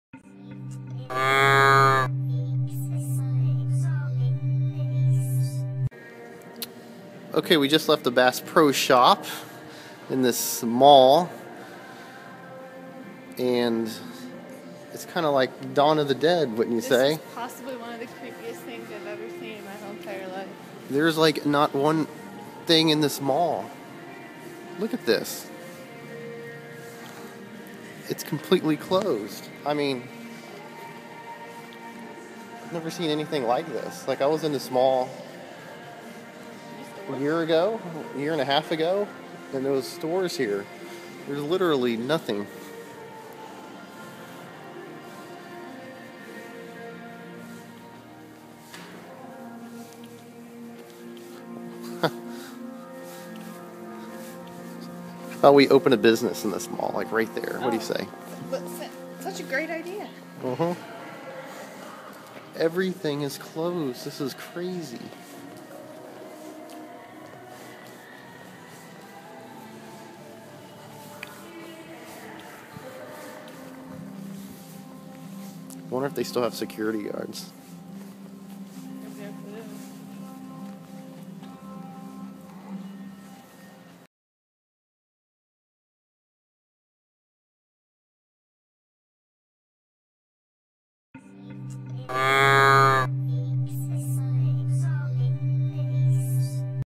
Out and About - The Worst Mall in the History of Malls
Cincinnati Mall in Springdale, Ohio. This would be a great place to film a movie, but not open a mall store.